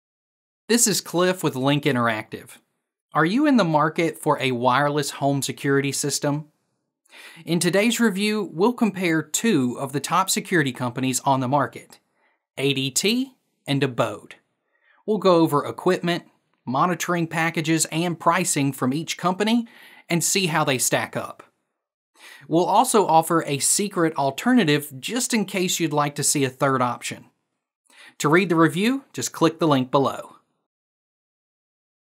Abode vs ADT Home Security: An Expert Comparison
Today we will be comparing ADT with a new DIY home security system called Abode and see how they match up. Let’s dive right in!